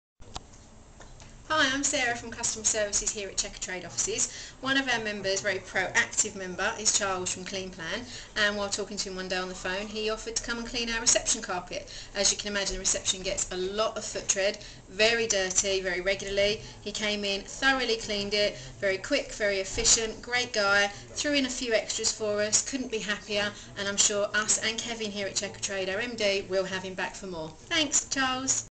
Carpet Cleaning at Checkatrade Headquarters, Selsey
Carpet and Upholstery cleaning at Checkatrade Headquarters by Clean Plan!